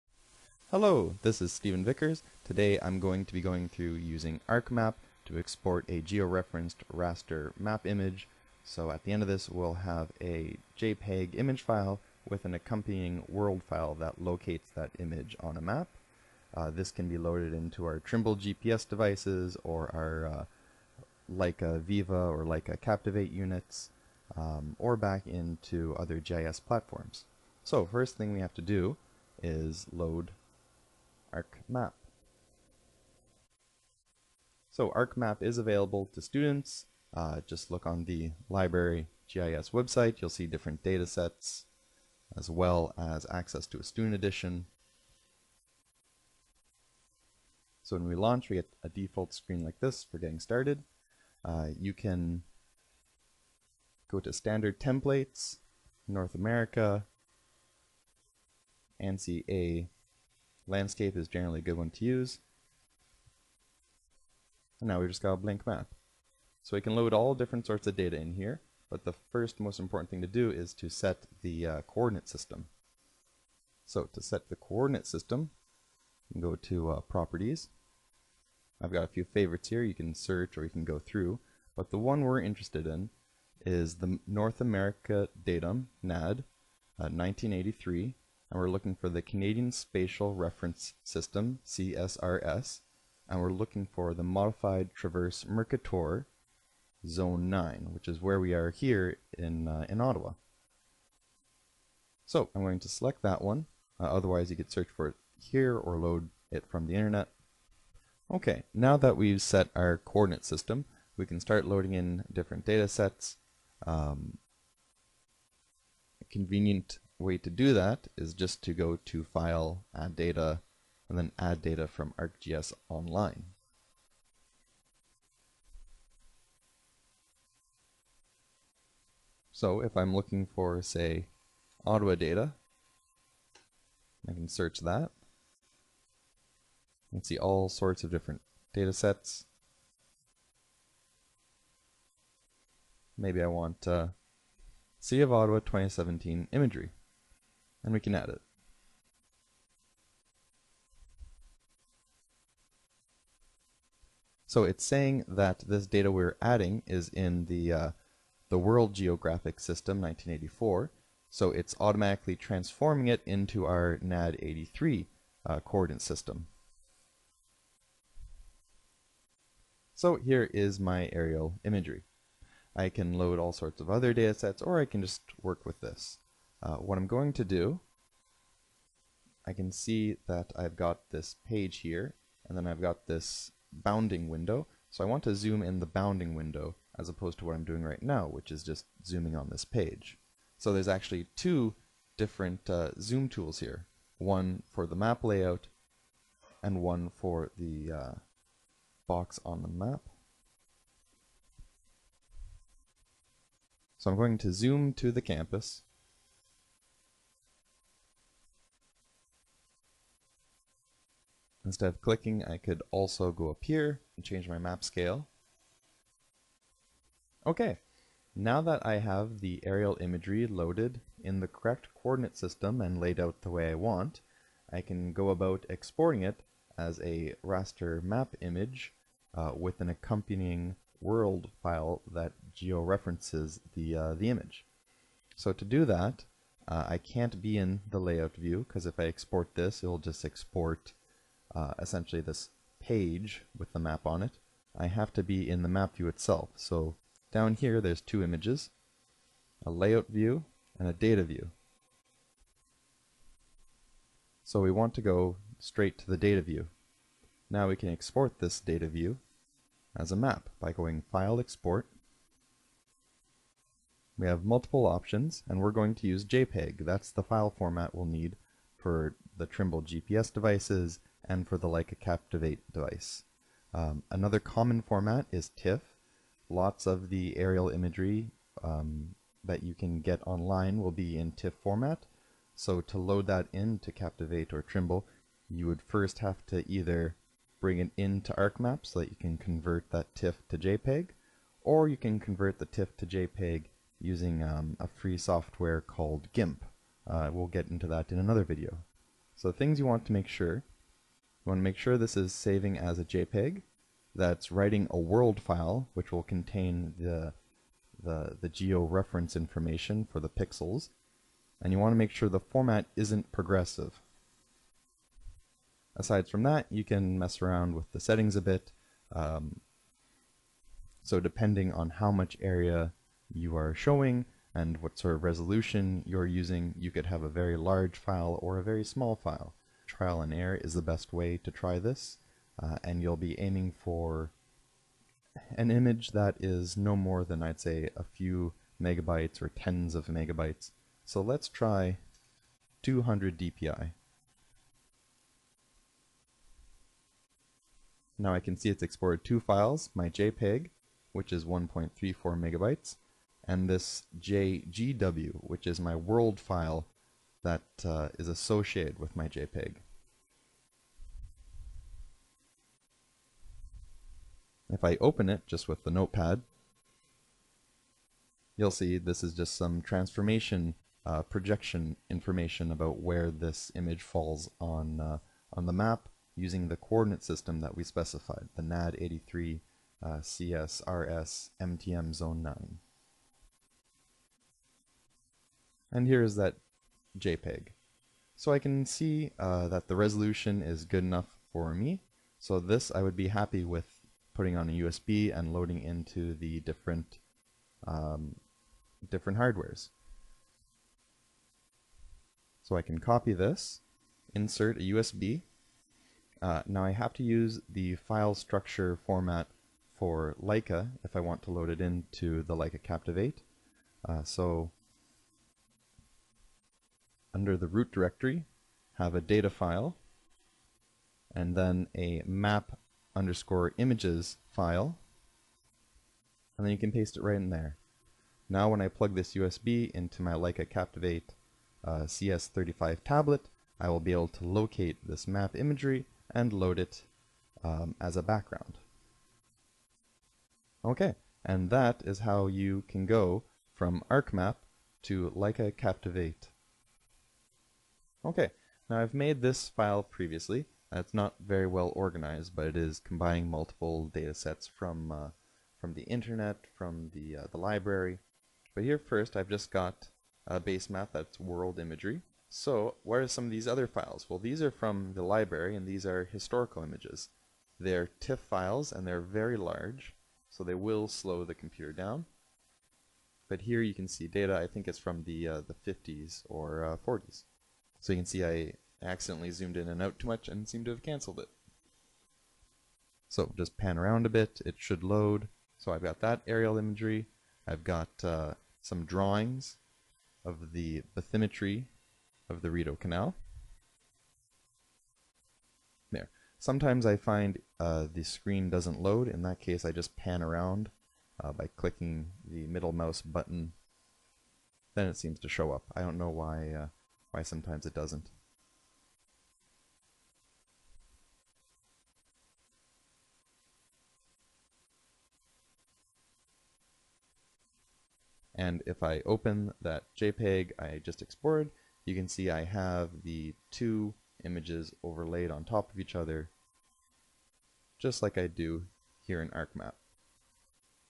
ArcMap Exporting Map Image & World File (.JPG/.JGW)
A workflow showing how to export a .JPG and .JGW from ArcMap for later use in other software as background imagery (such as Leica Captivate or Trimble GPS devices).